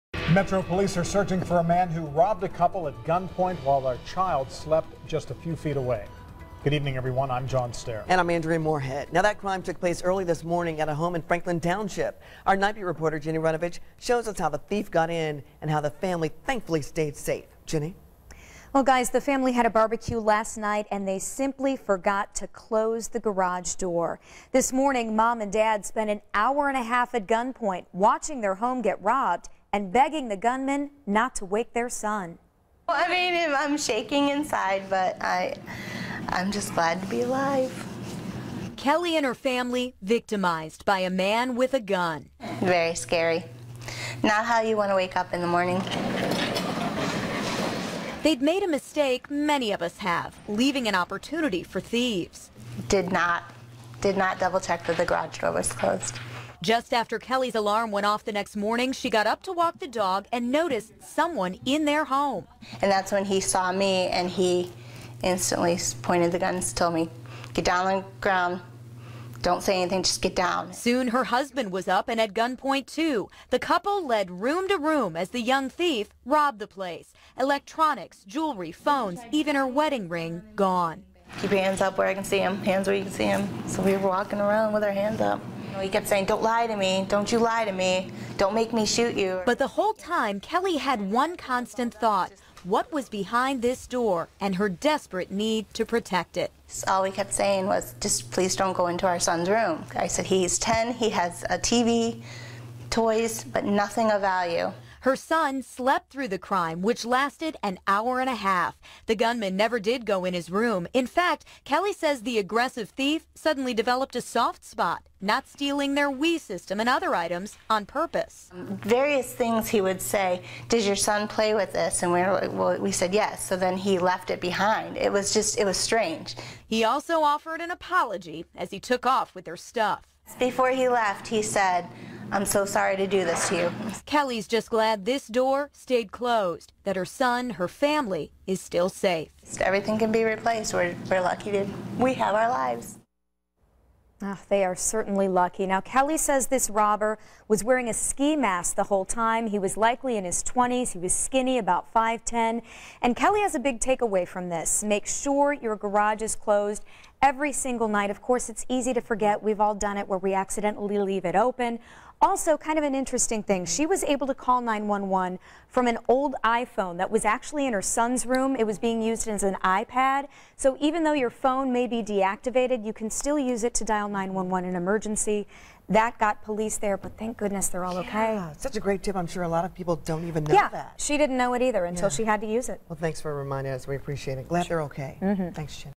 Couple robbed at gunpoint
An Indianapolis couple was robbed at gunpoint in their home, while their young son slept just feet away.